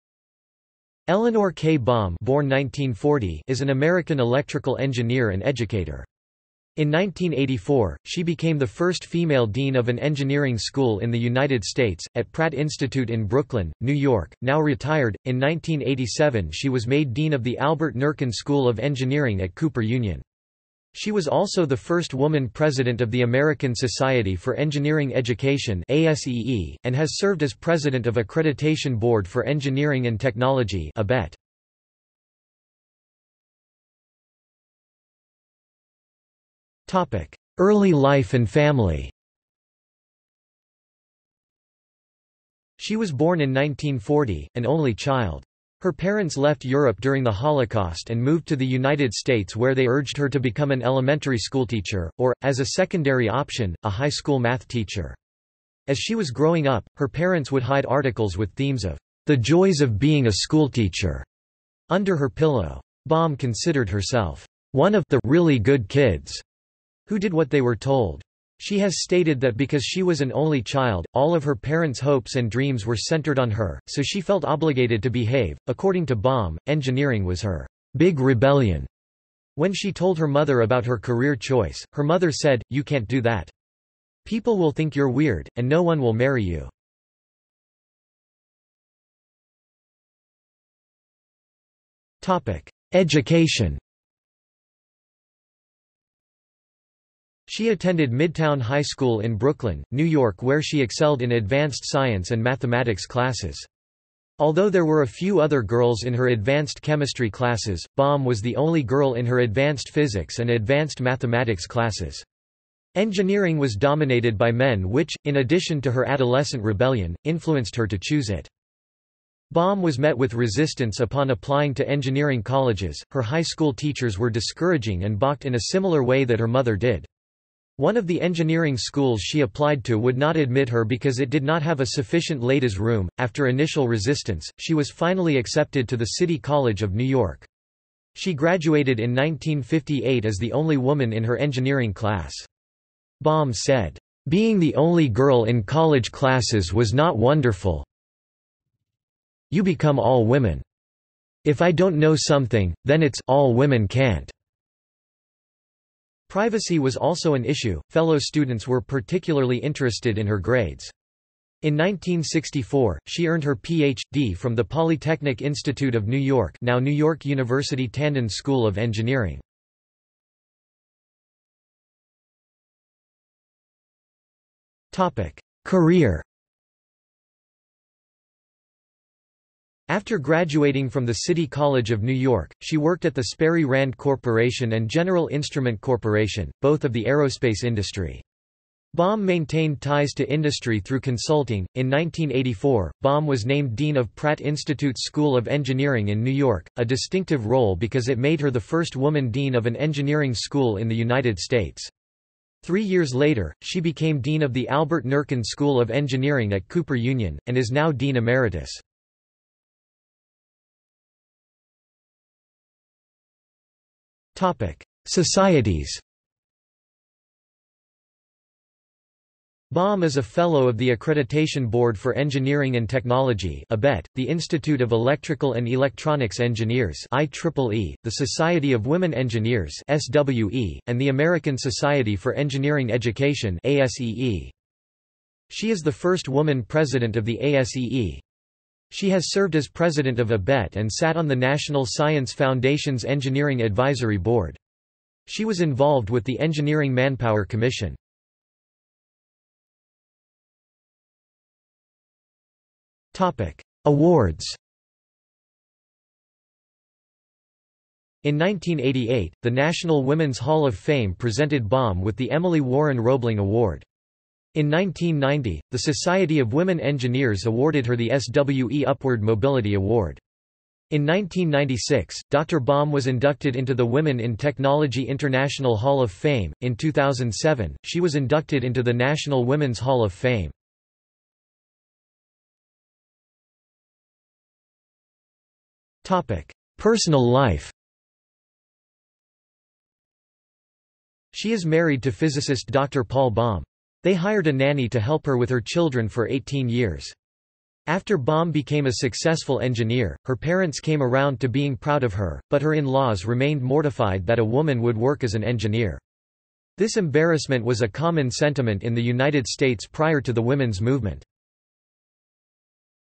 Eleanor K. Baum | Wikipedia audio article
This is an audio version of the Wikipedia Article:
Eleanor K. Baum

00:00:33 1 Early life and family
00:01:28 2 Education
00:02:51 3 Career
00:03:30 4 Societies
00:04:09 5 Awards
00:04:42 6 Personal life

- Socrates

SUMMARY
Eleanor K. Baum (born 1940) is an American electrical engineer and educator. In 1984, she became the first female dean of an engineering school in the United States, at Pratt Institute in Brooklyn, New York.Now retired, in 1987 she was made dean of the Albert Nerken School of Engineering at Cooper Union. She was also the first woman president of the American Society for Engineering Education (ASEE), and has served as president of Accreditation Board for Engineering and Technology (ABET).